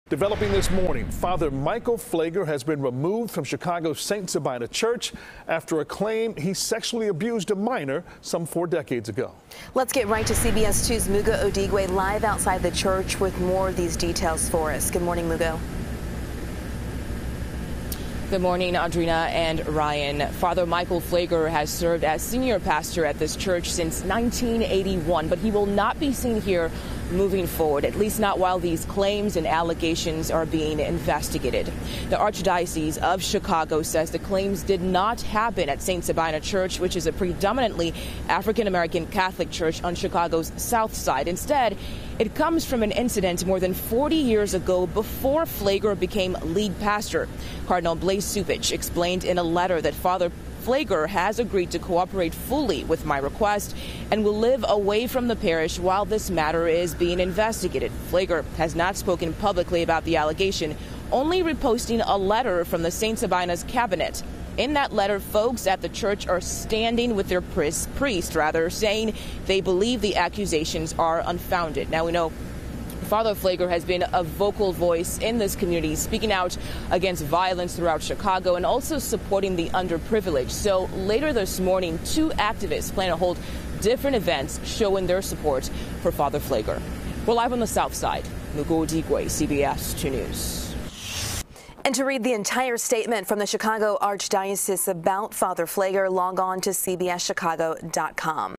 Father Michael Pfleger Faces Allegation Of Sexual Abuse Of Minor From More Than 40 Years Ago
Father Michael Pfleger is stepping away from St. Sabina due to a sexual abuse allegation of a minor more than 40 years ago.